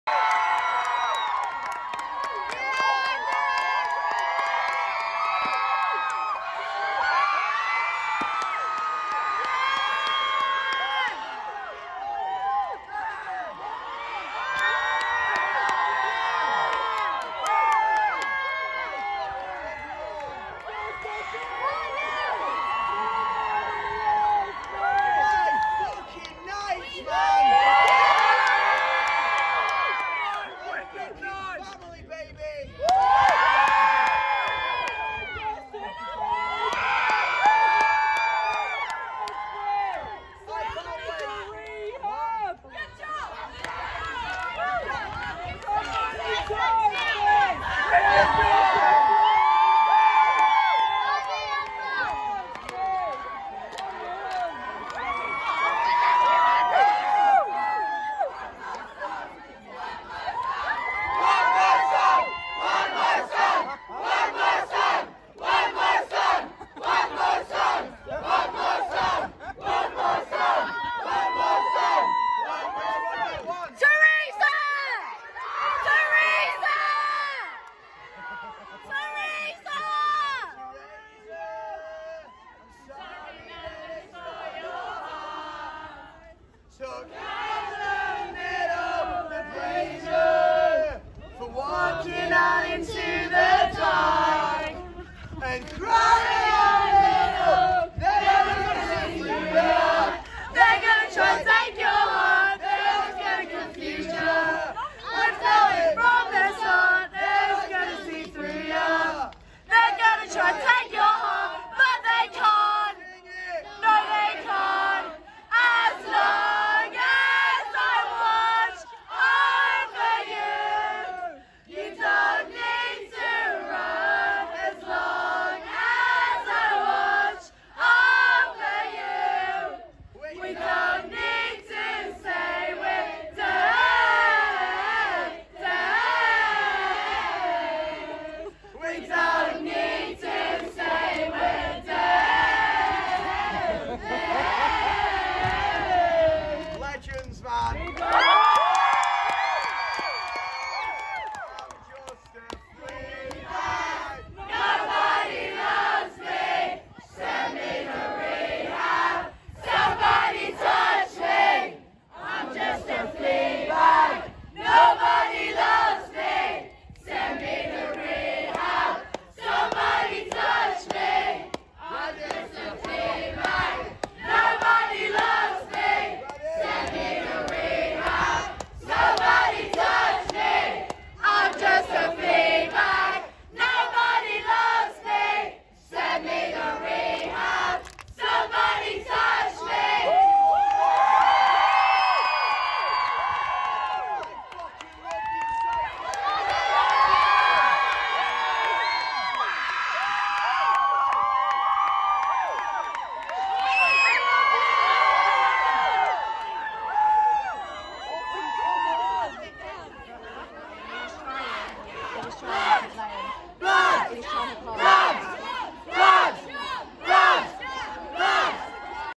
Yungblud after gig singalong
O2 Kentish Town impromptu singalong with fans.